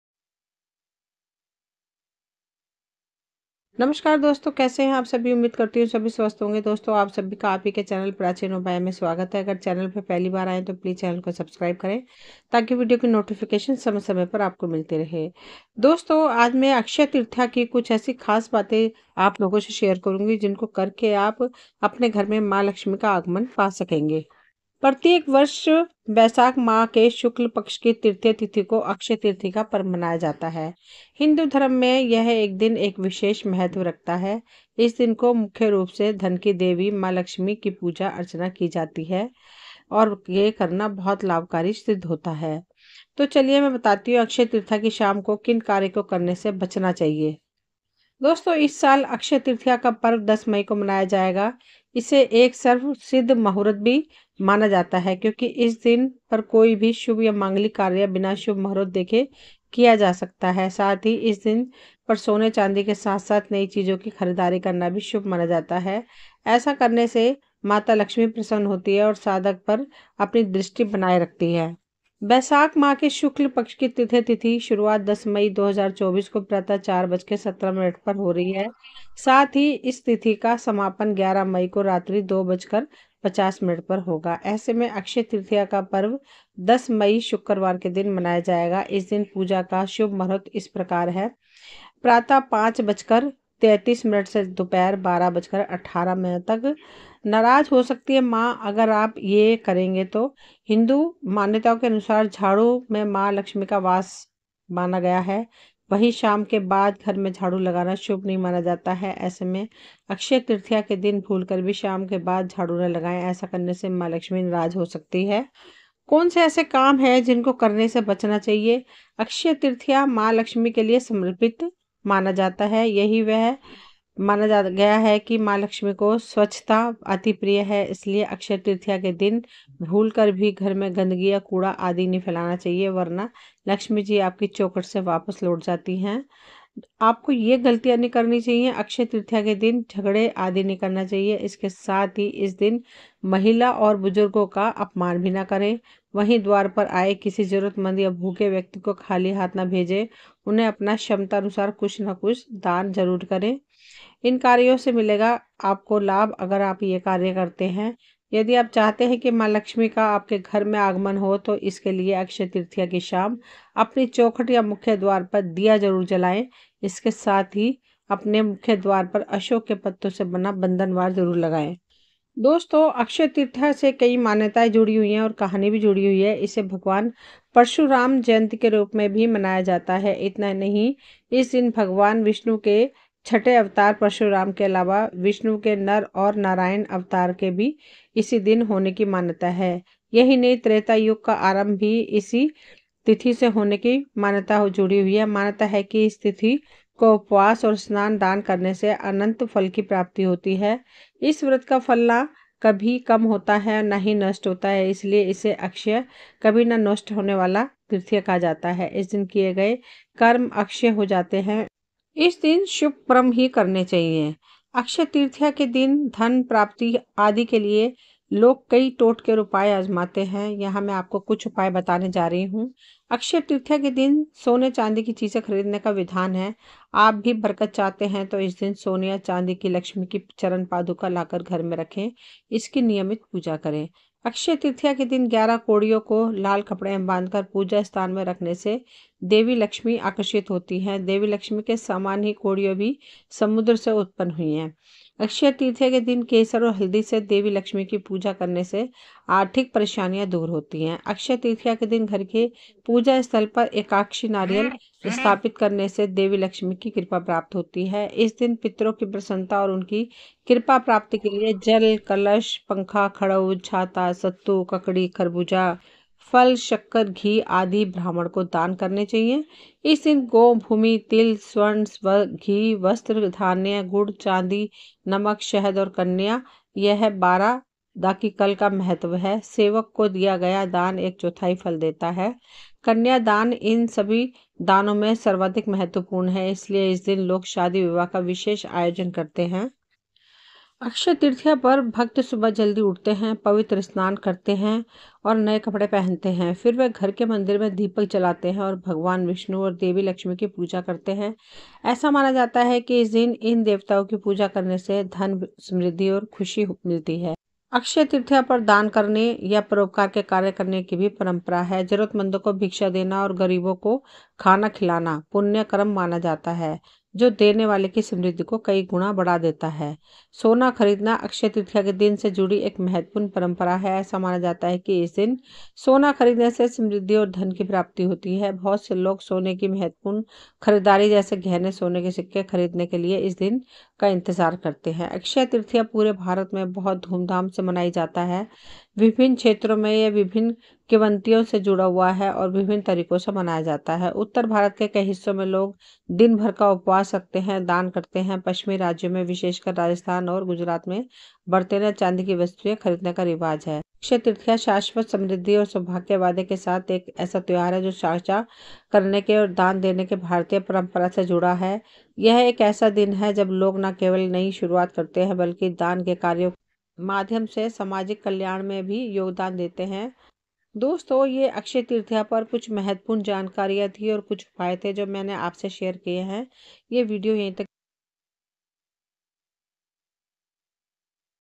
Akshaya Tritiya 2024: अक्षय तृतीया के दिन न करें ये काम, घर में नहीं होगा मां लक्ष्मी का वास
Akshaya Tritiya 2024: अक्षय तृतीया के दिन न करें ये काम, घर में नहीं होगा मां लक्ष्मी का वास l

अक्षय तृतीया 2024 कब है, नियम विधि, एवं उपाय Akshaya Tritiya 2024